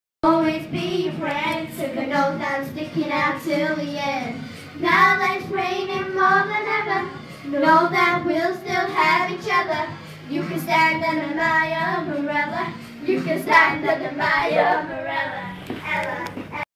Chloe and jodie
Chloe and Jodie doing a sing song on New Years eve